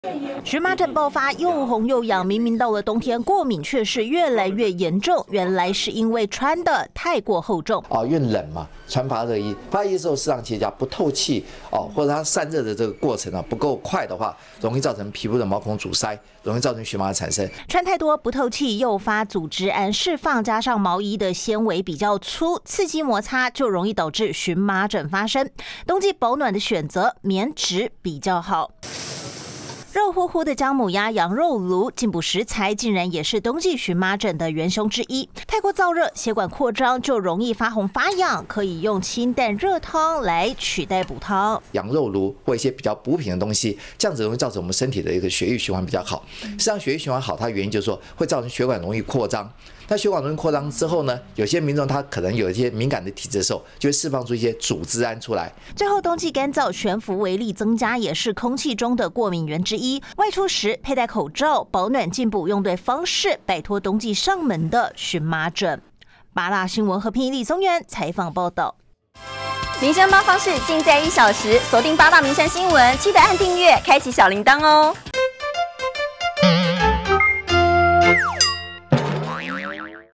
天氣冷「蕁麻疹」爆發 盤點3主因 |【民生八方事】| 2023122507
近日氣溫變化大，也有不少人發生蕁麻疹，奇癢難耐。醫師證實門診病患確實增加近一成，冬天確實容易造成蕁麻疹，但其實氣溫驟變是間接原因，主因包括飲食吃太補、穿著太厚重，毛衣質料刺激等，加上冬季乾燥懸浮微粒增加，都是造成冬季蕁麻疹的主因。